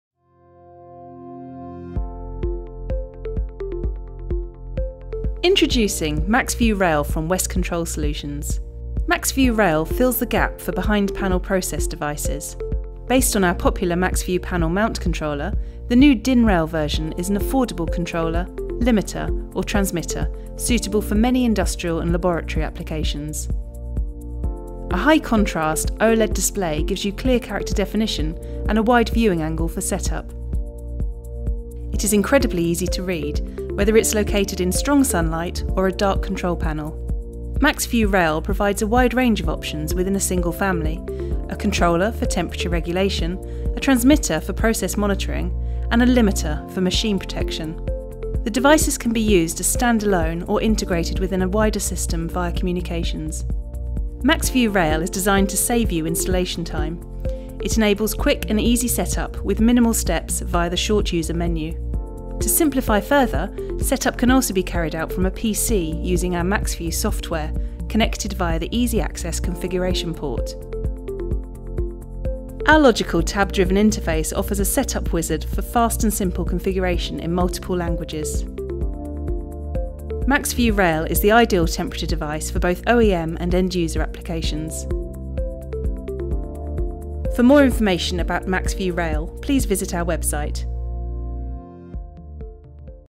Introduction to MAXVU Rail
MAXVU Rail is a compact DIN-rail mount temperature controller/transmitter and limiter range for general industrial and scientific equipment requiring a reduced back panel footprint, centralized control or minimal front panel components.